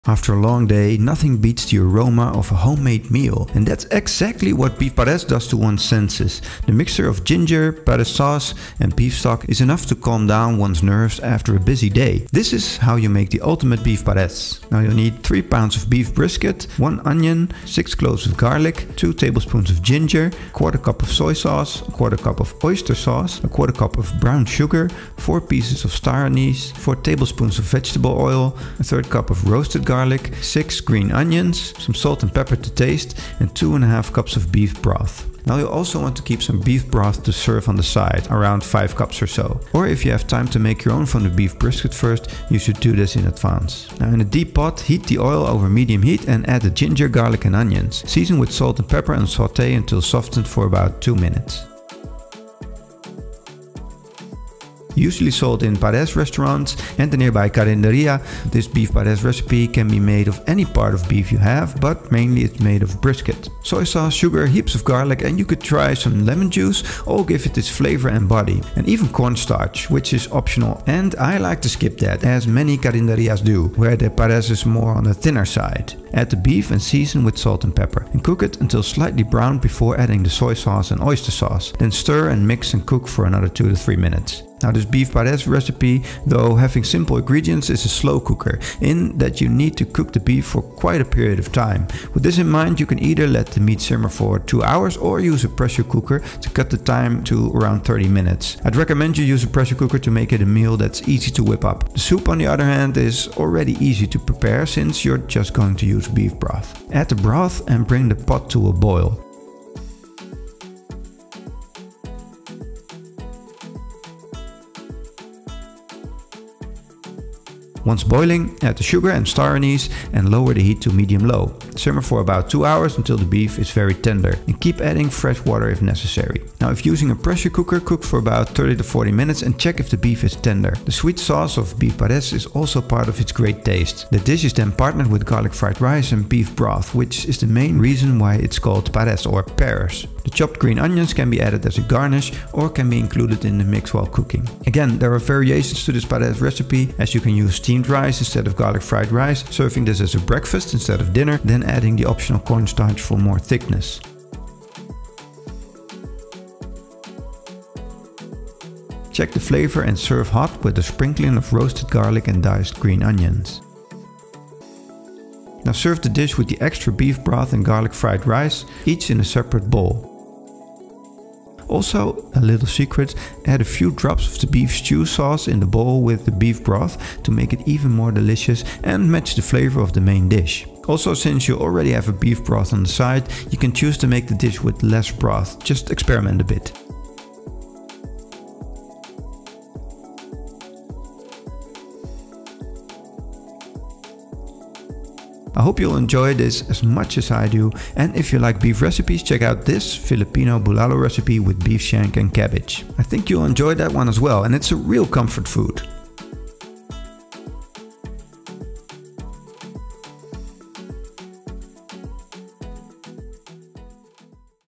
Filipino Beef Pares & broth recipe: Learn the secret tips to the perfect flavor
After a long day, nothing beats the aroma of a homemade meal, and that's exactly what Beef Pares does to one's senses.

The mixture of ginger, pares sauce, and beef stock is enough to calm down one's nerves after a busy day.

This is how you make the ultimate beef pares.

And also, we've got our snackable mascot up for sale now on your favorite sized t-shirts.

In this video:
00:00 How the beef pares will look
00:13 Ingredients
00:43 Saute Onions
01:16 Add the beef
01:47 Add the broth
02:27 Serve with rice and broth